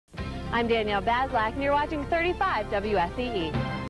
WSEE Station ID 2003
This is WSEE's station ID from 2003
WSEE is located in Erie, PA